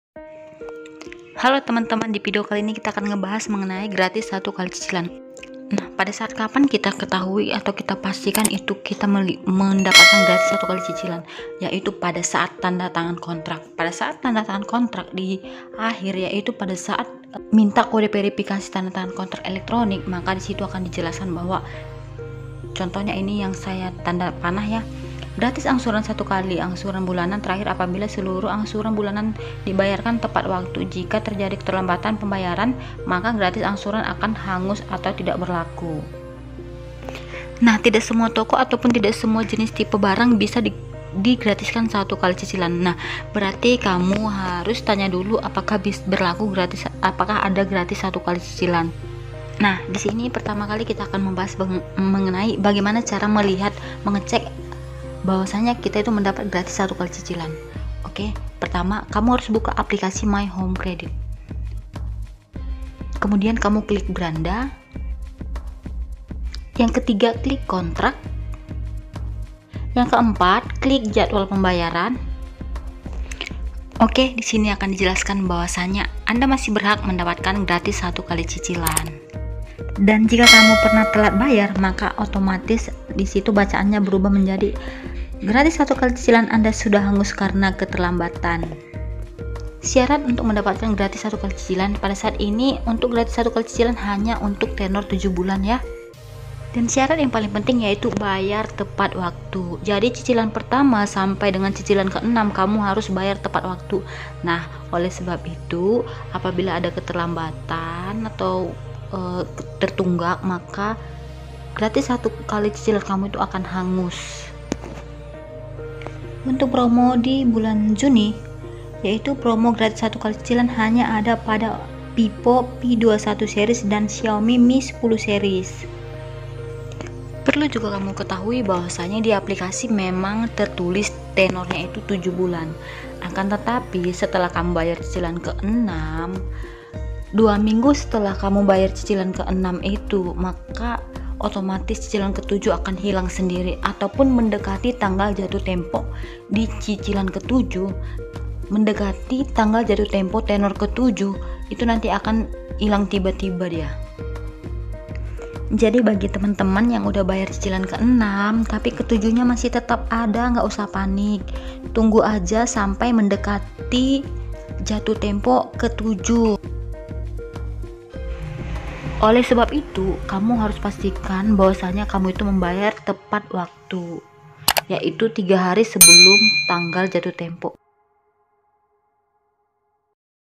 Syarat Mendapat GRATIS 1X Cicilan di Home Credit || Cara mendapat Gratis 1X CICILAN HOME CREDIT
Syarat Mendapat GRATIS 1X Cicilan pastikan kamu bayar tepat waktu untuk cicilan pertama sampai ke 6 maka akan gratis cicilan ke 7.
Nah Gak semua Type Atau Store teesedia cicilan Gratis 1x, Perlu kamu tanyakan ke petugas Home Credit nya apakah berlaku gratis 1x Cicilan, dan bisa kamu Buktikan dikontrak kamu saat tandatangan kontrak.
Ketika Meminta Kode Tandatangan Kontrak Elektronik, dihalaman tersebut ada terlampir Persetujuan Syarat Gratis 1x Cicila, Dan Kamu Harus Centang dibagian peesetujuan tsb Untuk menyelesaikan Tandatangan Kontrak kamu.